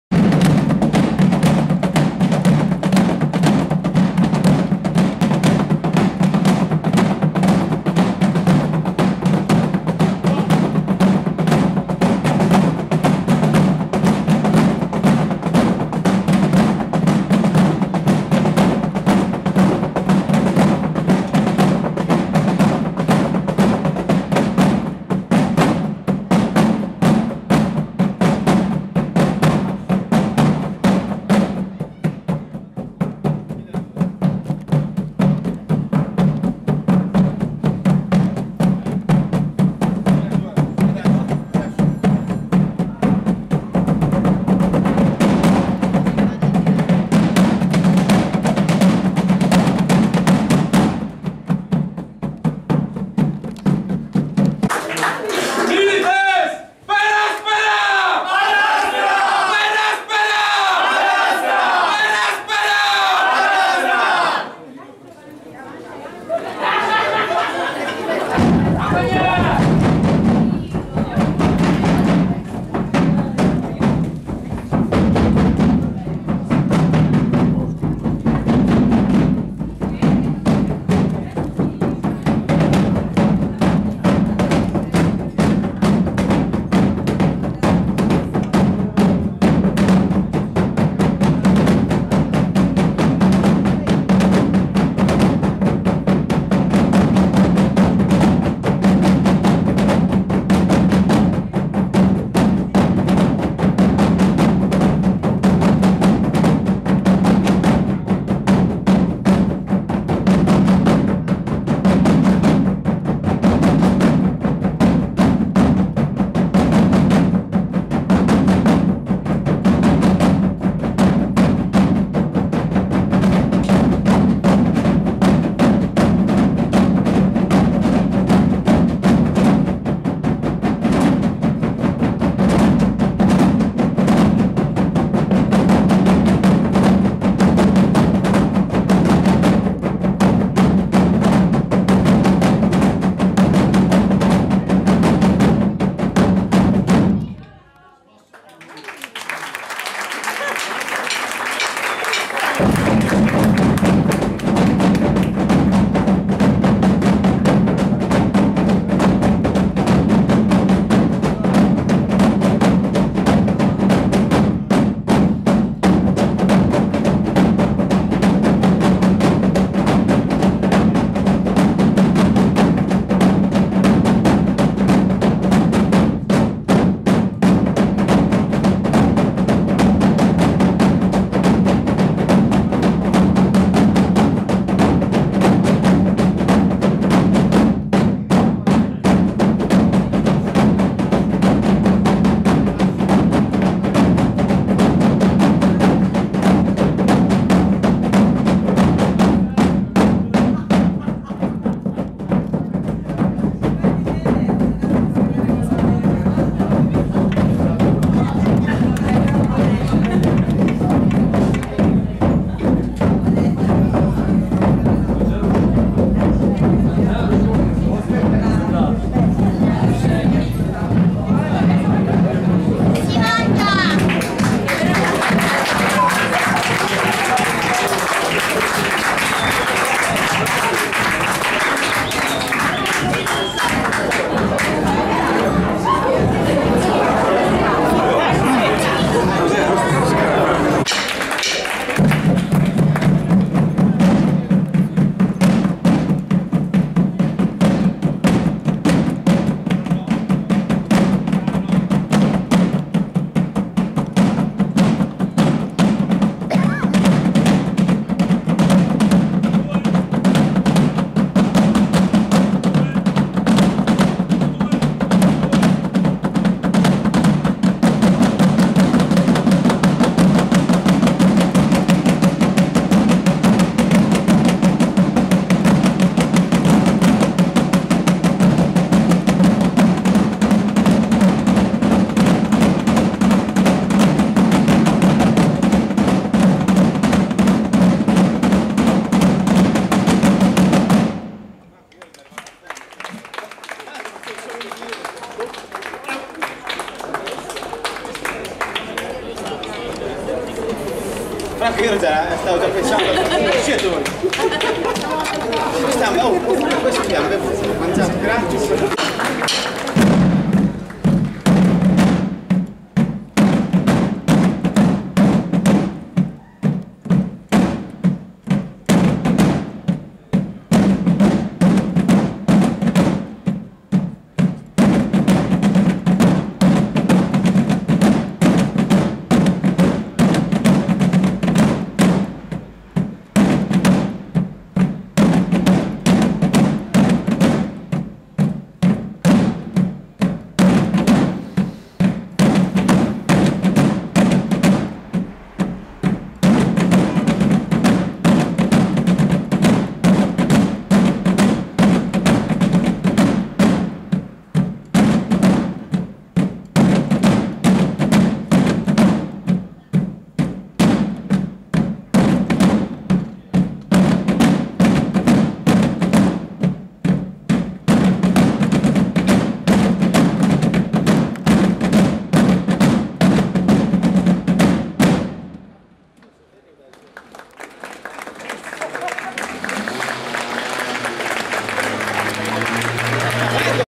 LECTISTERNIVM tamburi medioevali Brisighella
I tamburi di brisighella hanno suonato al ritmo della vita durante il LECTISTERNIVM tenuto alla " Taberna Boaria " di Ravenna, dentro e fuori e io ho messo delle immagini della Davida, il monumento dedicato alla pace, quando era in fase di nascita.
Montellik